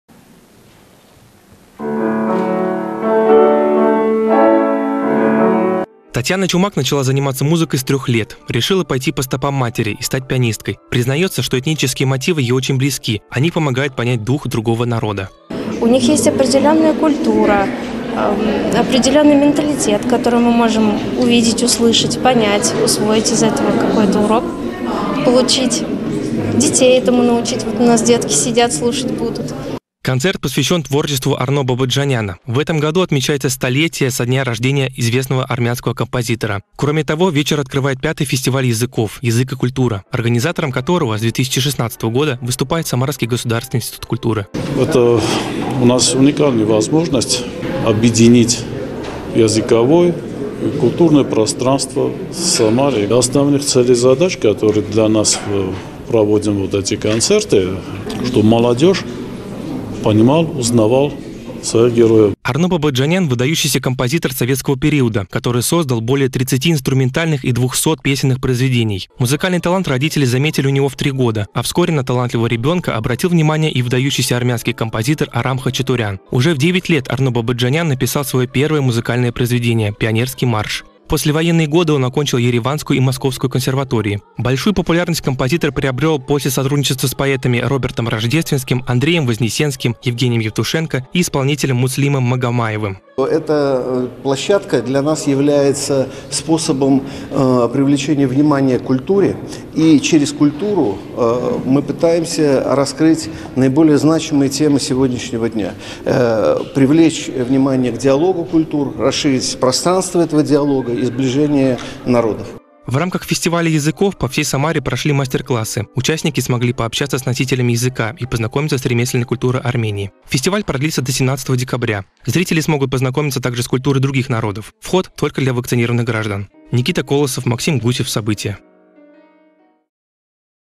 В Самаре прошел концерт песен Арно Бабаджаняна
Познакомиться с культурой народа через музыку. В  Самарском государственном институте культуры прошел концерт в честь 100-летия известного советского композитора Арно Бабаджаняна. Удивительные мелодии услышала наша съемочная группа.